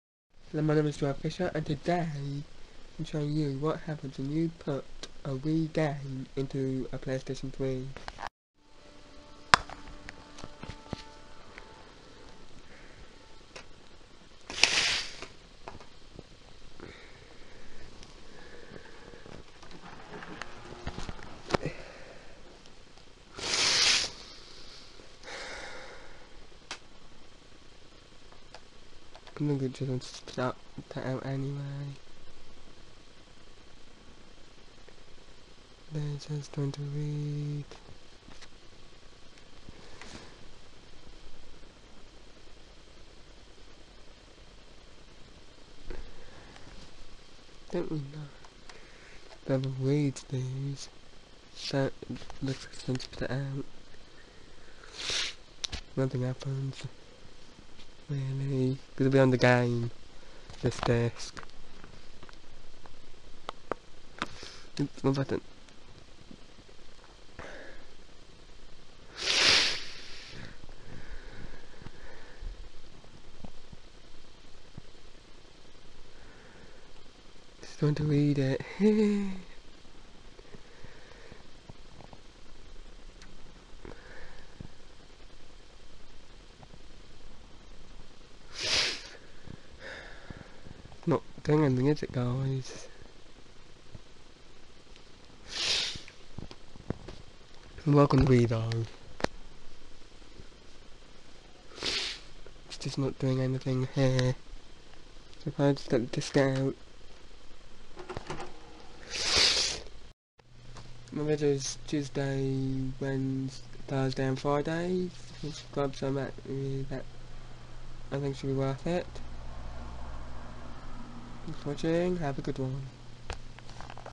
What Happens When you put a Nintentdo Wii disc into a PS3
This is what happens when you put a Nintendo Wii game into a PS3 system.